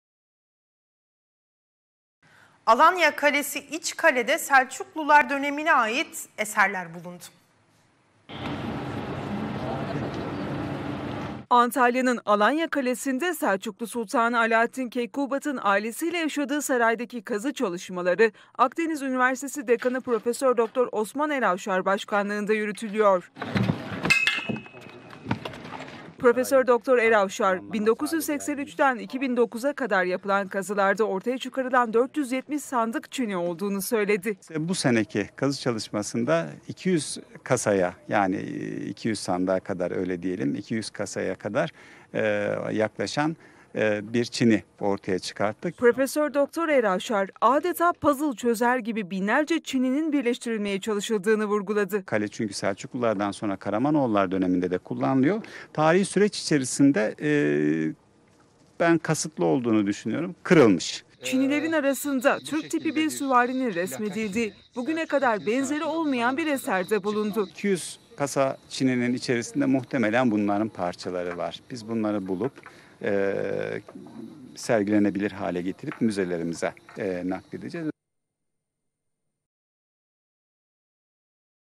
Selçuklu Dönemine Ait Eser Bulundu
Alanya Kalesi’nde, Akdeniz Üniversitesi Güzel Sanatlar Fakültesi Dekanı Prof. Dr. Osman Eravşar başkanlığında yürütülen Selçuklu dönemine ait kazı çalışmalarında bu yıl son derece önemli, ünik (eşi olmayan) bir parça tespit edildiğini belitti. Prof. Dr. Eravşar, "Bu şekilde bir plaka çini, Selçuklu çini sanatında bu zamana kadar çıkmamıştı" dedi.